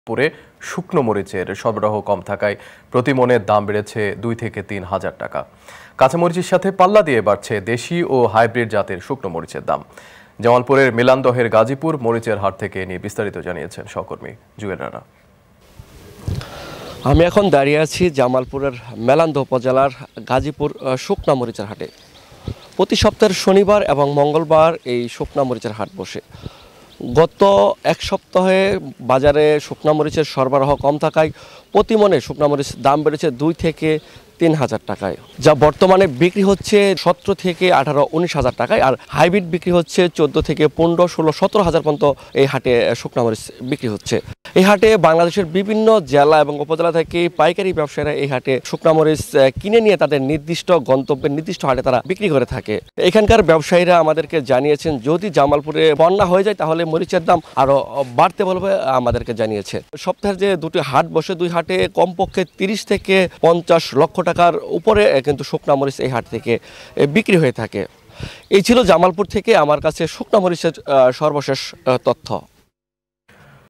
জামালপুরে শুকনো মরিচের সরববাহ কম | Jamalpur News | Chili Price | Bazar Update | Ekhon TV
জামালপুরে শুকনো মরিচের সরববাহ কম থাকায় প্রতিমণে দাম বেড়েছে ২ থেকে ৩ হাজার টাকা। কাঁচা মরিচের সাথে পাল্লা দিয়ে বাড়ছে দেশি ও হাইব্রিড জাতের শুকনো মরিচের দাম।

Our Office Address:
EKHON TV
City Park Lane
19, Hatkhola Road, Wari
Dhaka-1203
Bangladesh